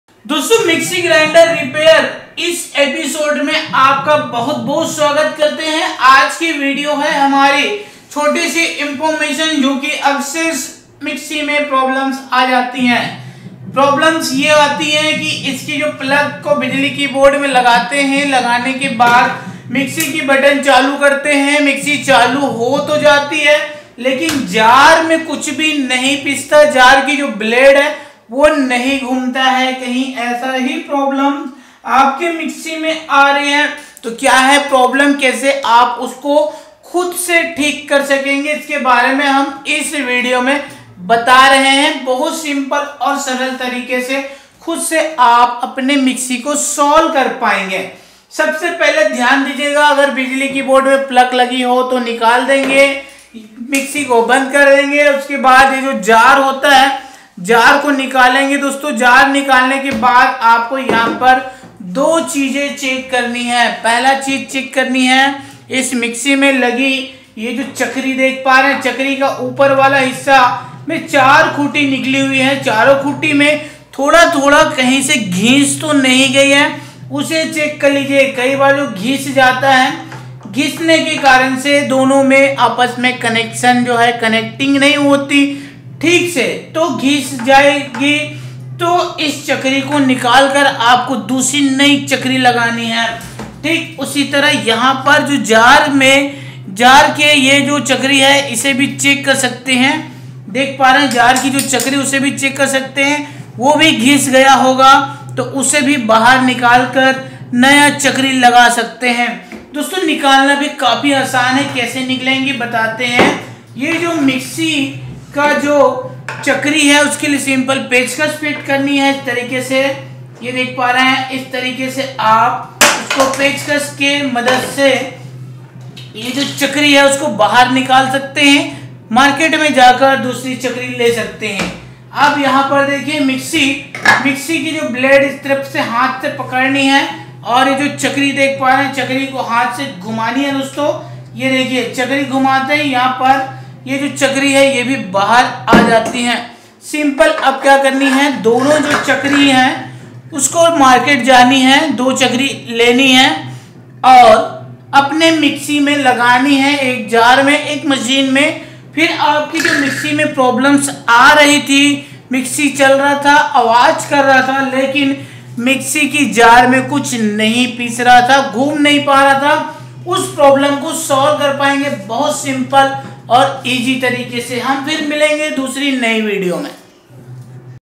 मिक्सर ग्राइंडर रिपेयरिंग आवाज आ रहा है घूमता नहीं ठीक करे how to repair mixer grinder sound problem
मिक्सर ग्राइंडर रिपेयरिंग, आवाज आ रहा है घूमता नहीं ठीक करने का तरीका | how to repair mixer grinder sound problem